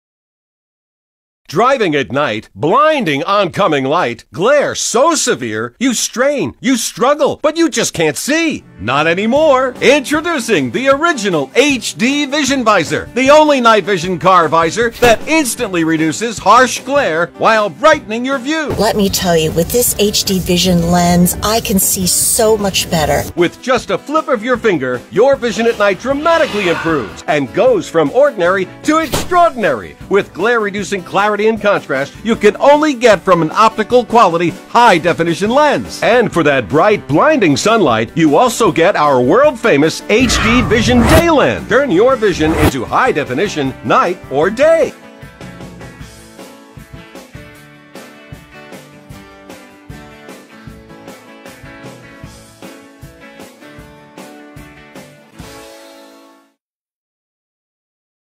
Car Sun Visor HD Anti-dazzle Clip-on Goggle
Stop the dazzling of the headlights or sun!

It is important to protect your eyes from harmful elements including ultraviolet rays and headlights, especially when you are driving.